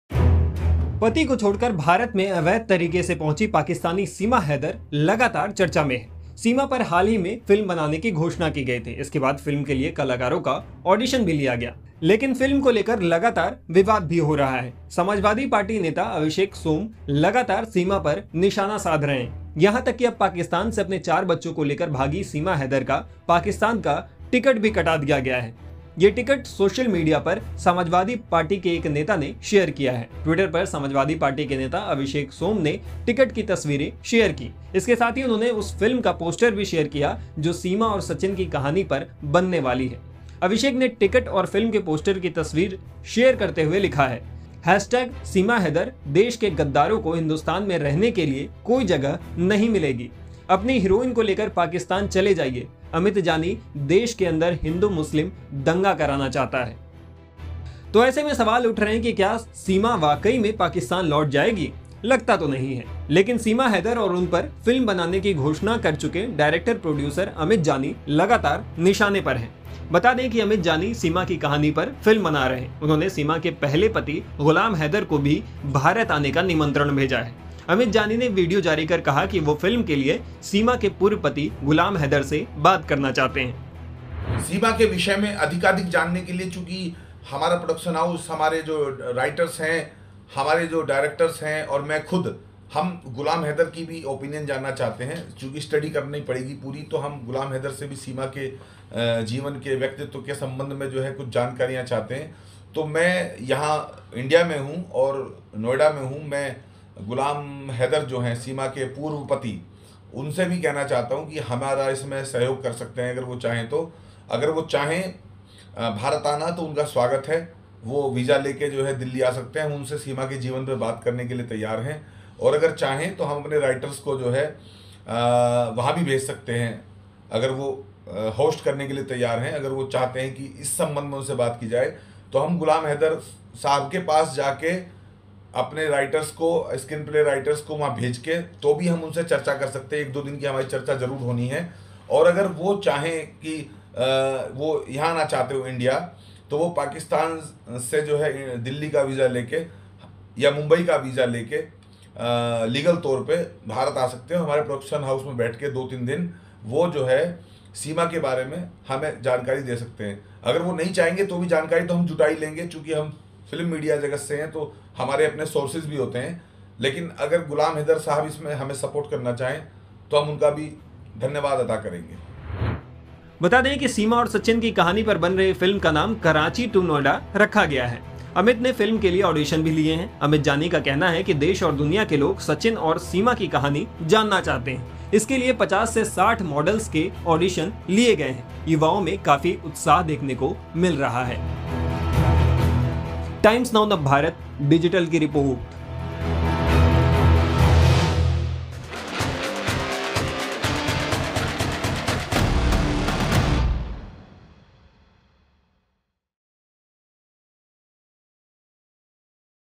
Seema Haider का कटा फ्लाइट टिकट, Sachin को छोड़ लौटेगी Pakistan ? | Latest News | Hindi News
पति को छोड़कर भारत में अवैध तरीके से पहुंची पाकिस्तानी सीमा हैदर लगातार चर्चा में है. सीमा पर हाल ही में फिल्म बनाने की घोषणा की गई थी. इसके बाद फिल्म के लिए कलाकारों का ऑडिशन भी लिया गया. लेकिन फिल्म को लेकर लगातार विवाद भी हो रहा है.